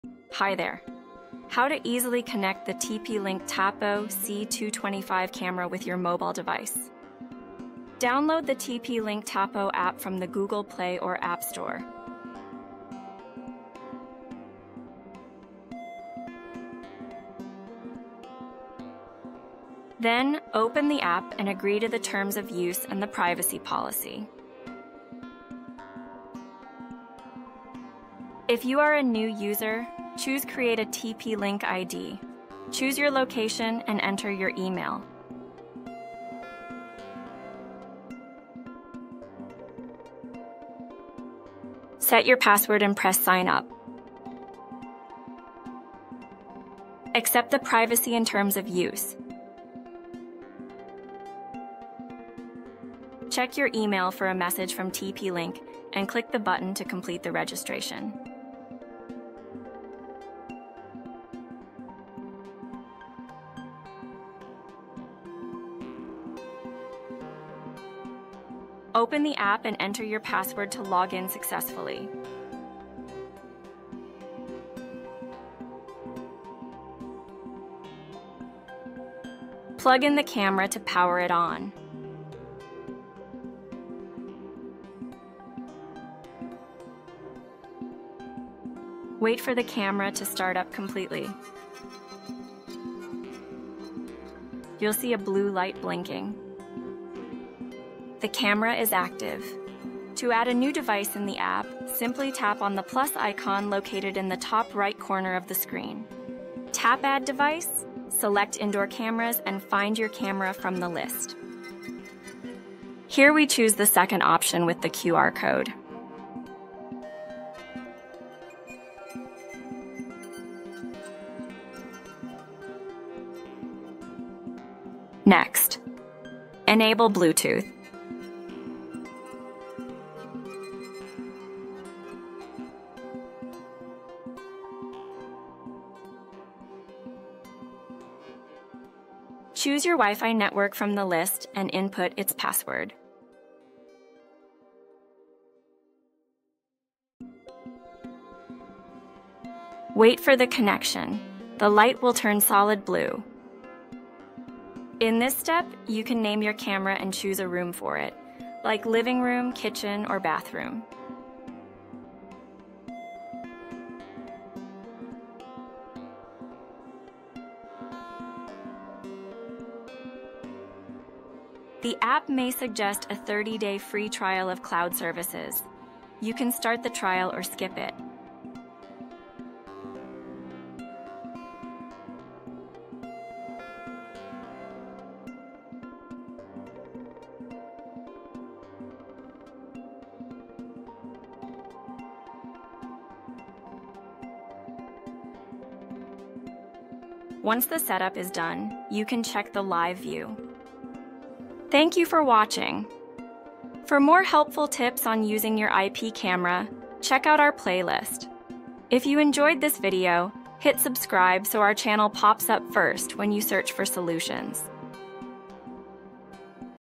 TP-Link Tapo C225: How to Connect. Easy WiFi Camera Connection Guide
Worried about setting up your TP-Link Tapo C225 camera? Relax! This video walks you through the entire connection process in simple steps. Get your camera online and start monitoring your home in no time.   Perfect for beginners!